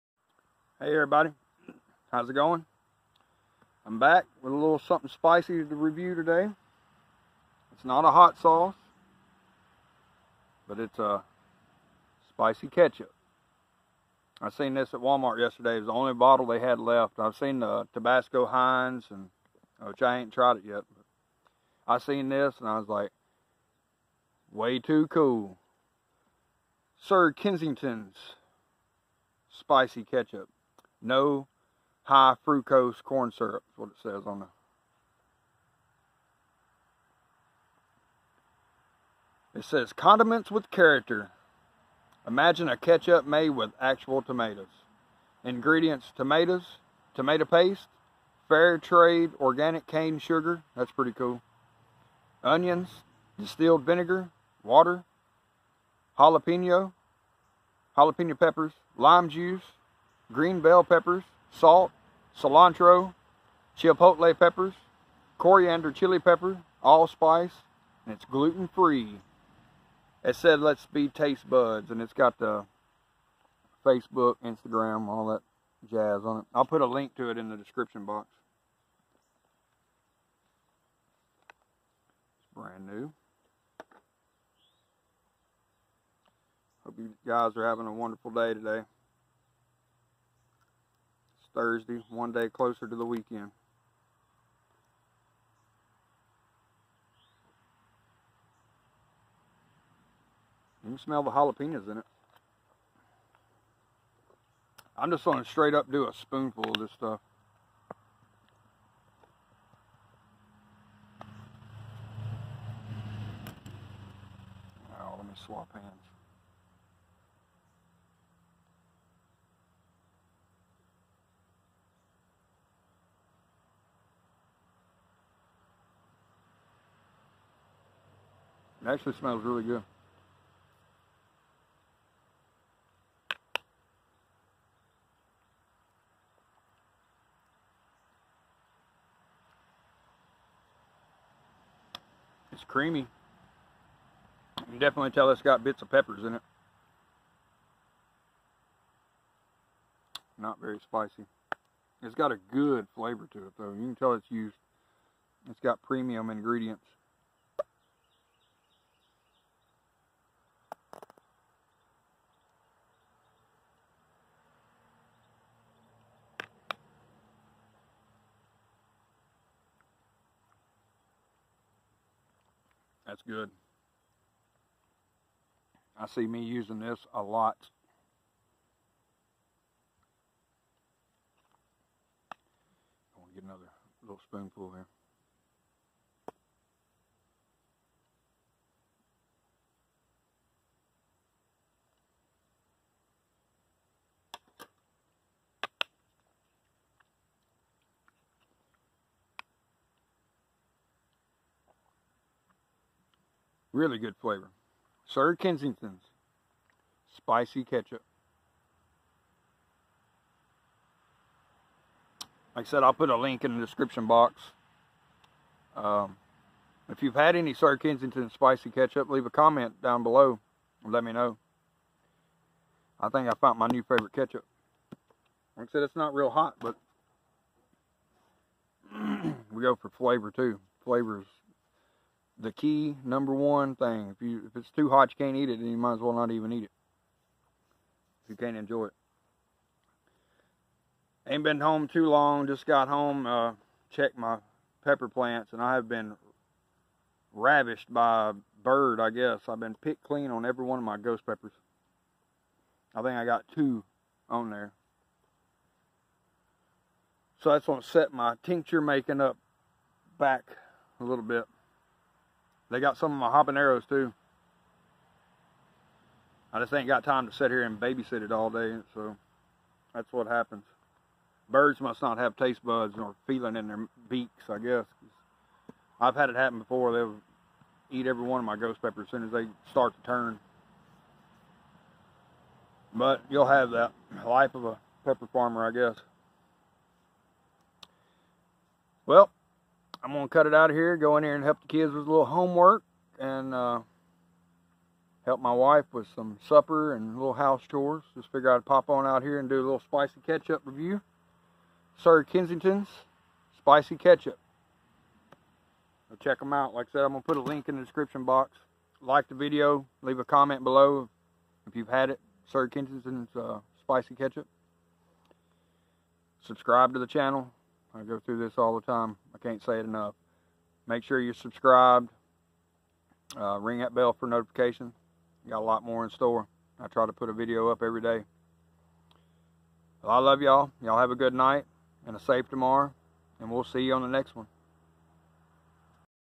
Sir Kensington's spicy ketchup
Spicy ketchup from Sir Kensington's. My new favorite ketchup. Not a lot of heat, but great flavor, with simple good ingredients. Thank you all!